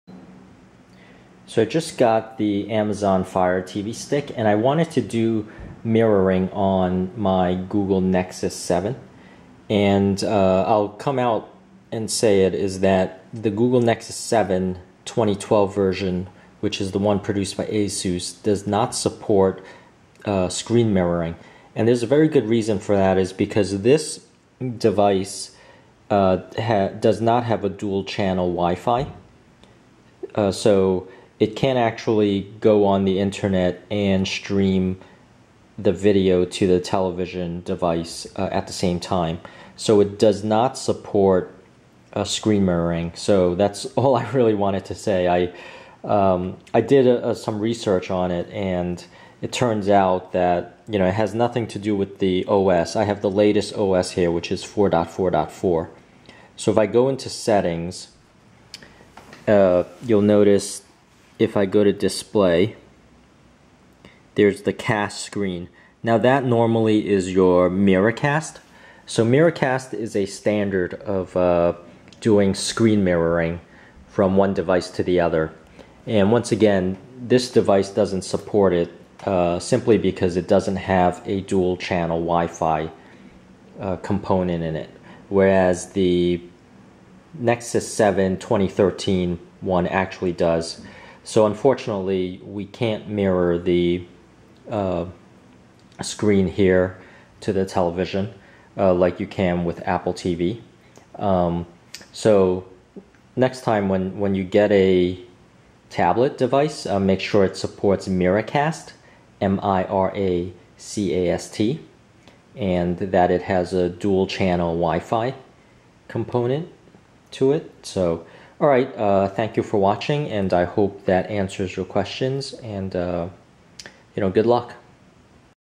Google Nexus 7 (2012) - MiraCast Not Supported on 1st Generation
Explaining why Google Nexus 7 2012 version does not support MiraCast, because it does not have the Hardware for it, specifically it does not have a Dual Band or Dual Channel WiFi component